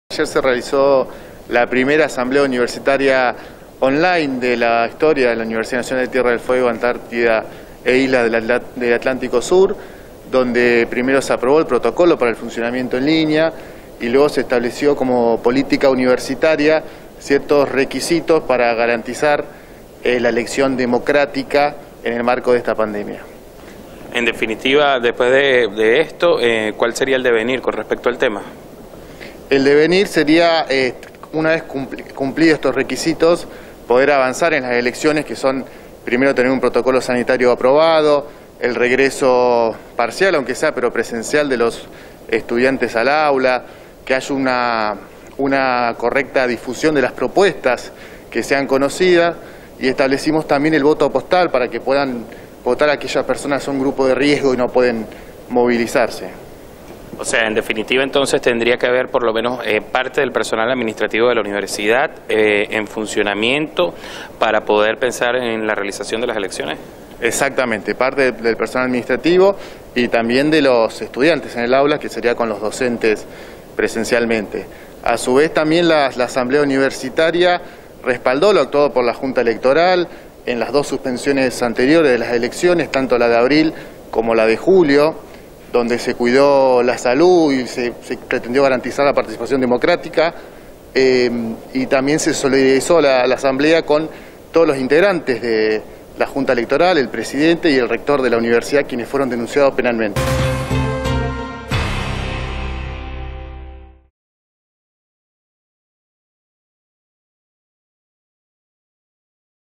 Elecciones en la Universidad de Tierra del Fuego
Se definío en Asamblea los requisitos para realizar las elecciones en la UNTDF, segùn la opinión de Gabriel Carol “Debe estar en funcionamiento parte del personal administrativo y esperar aprobación del protocolo por Nación”